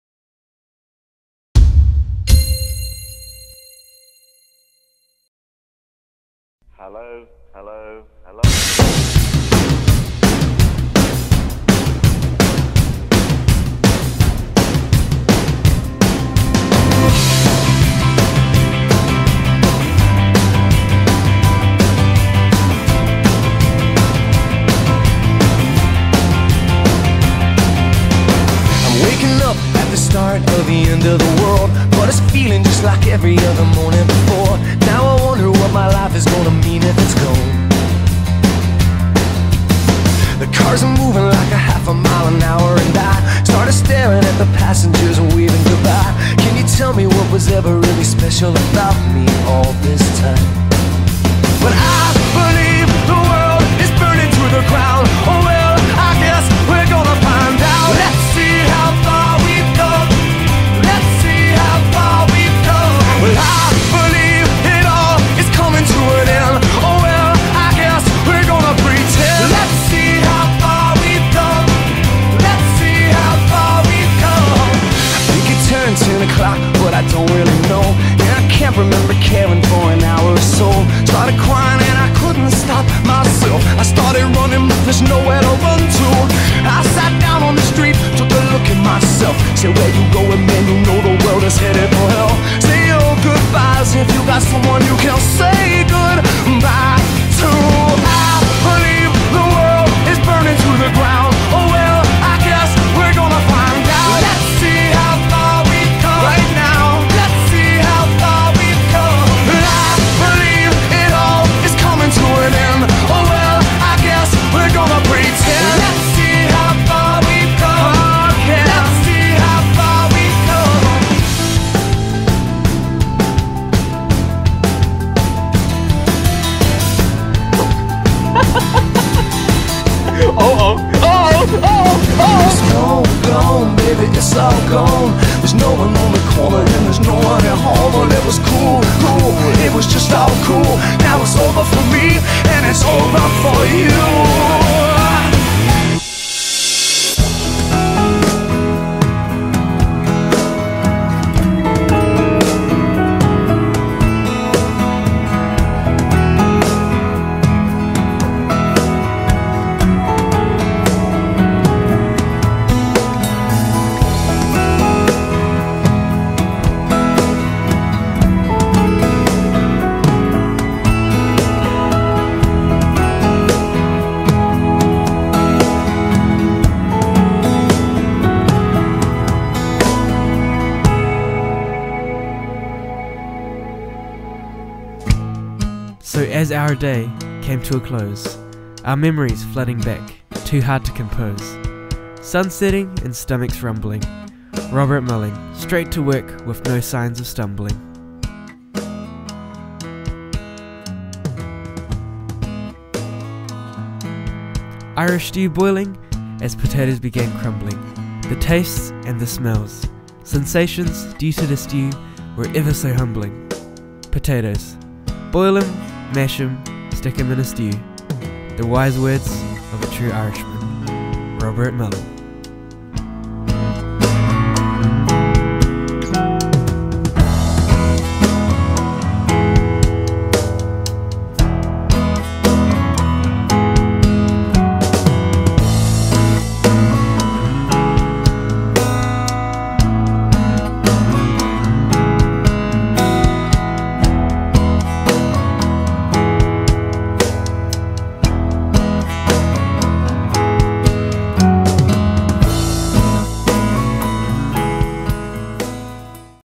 West Coast Journey - Day 1 - "An Eventful Day"
This is DAY 1 of a 5 day video diary of 5 friends spending 5 incredible days in the haven of which is Lake Brunner's Moana in the West Coast, New Zealand.